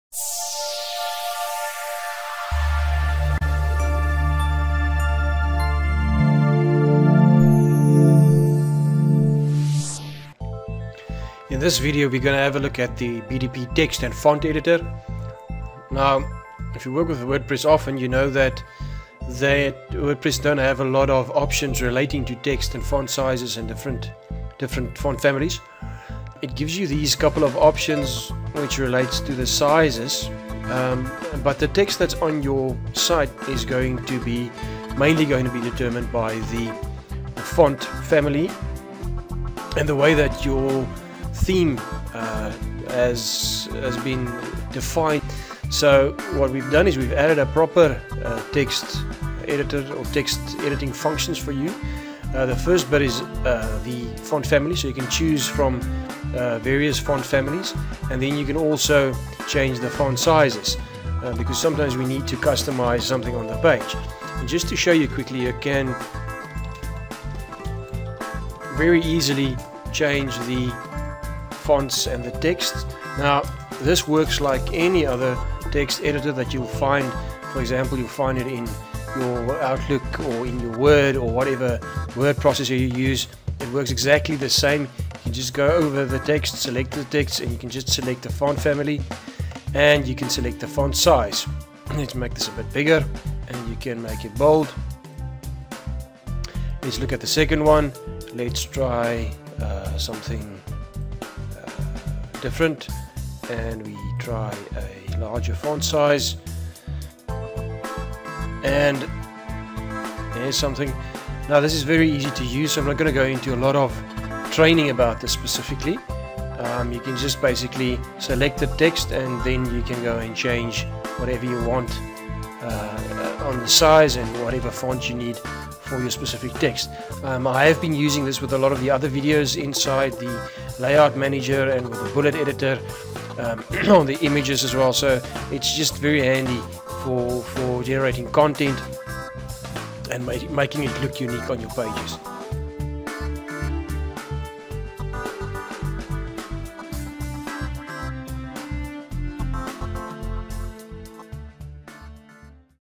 Best Design Plugin for Wordpress - Font Editor
Font editor for Wordpress

Make use of a proper text editor that has been missing to your WordPress dashboard.

Choose from 17 font styles and 23 size adjustments to make your text look the way it should and make sure your design look the way you want it to.